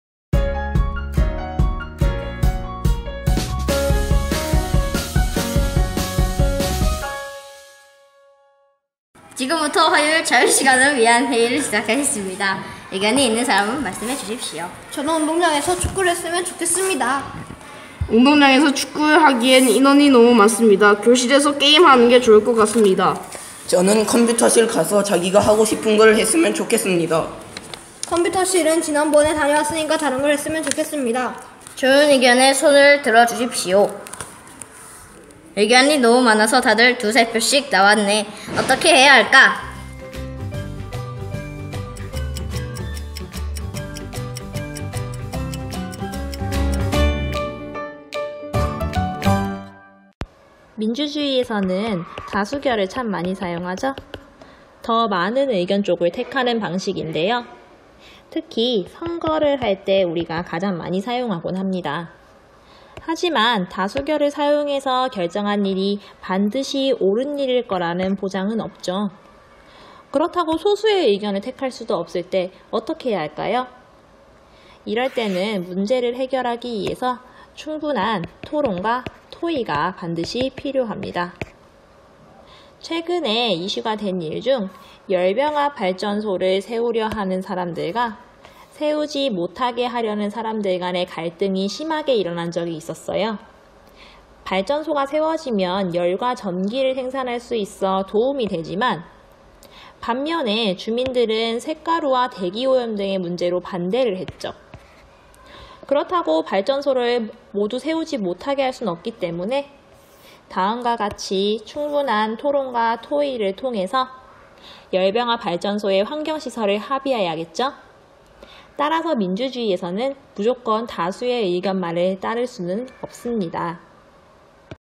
[쉽고 재미있게 Talk하는 사회이야기] ㅣ6학년ㅣ03ㅣ민주주의에서는 다수의 의견을 반드시 따를까
ㅣ6학년ㅣ03ㅣ민주주의에서는 다수의 의견을 반드시 따를까